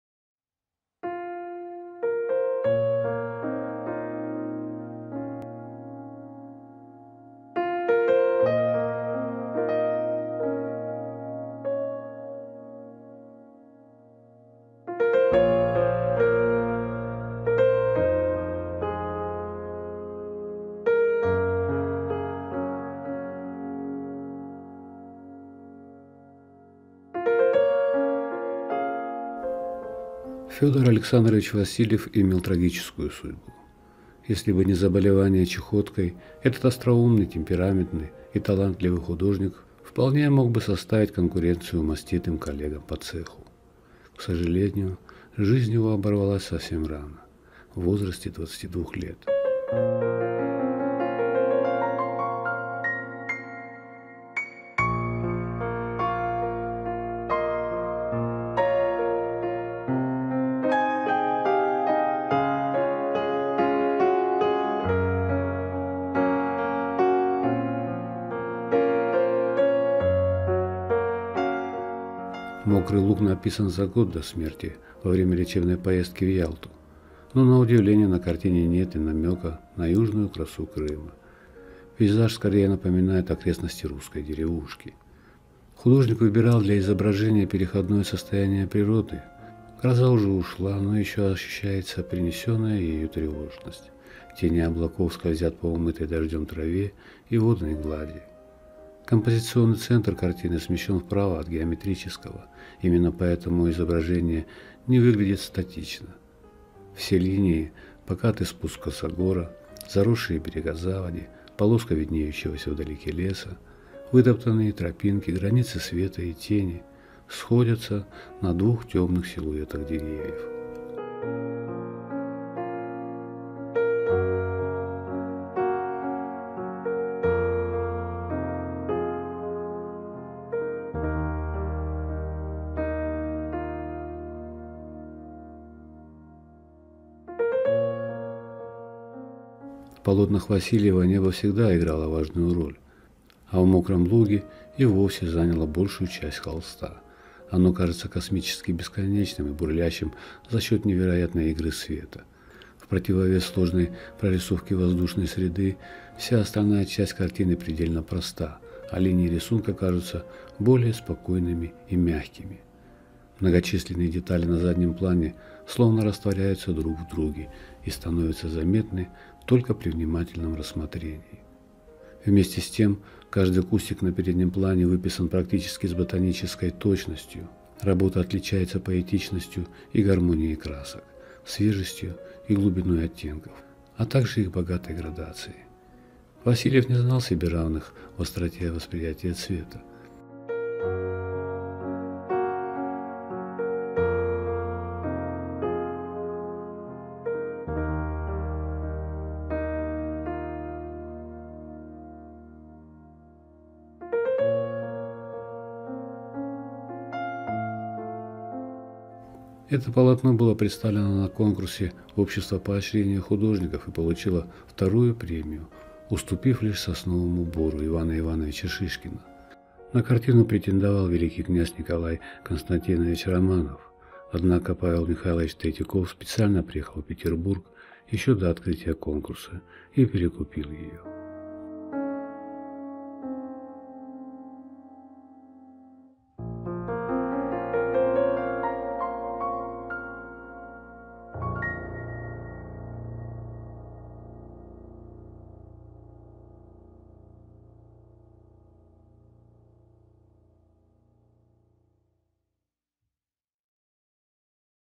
Шедевры русской живописи. Мокрый луг.
Картина "Мокрый луг" была написана
Фёдором Александром Васильевым за год до
смерти художника. Картина была написана в Ялте,
но больше напоминает окрестности русской деревни.

описание картины Мокрый луг:
Фёдор Александрович Васильев
картина Мокрый луг
Дата 1872 г.
техника
масло
холст
Размеры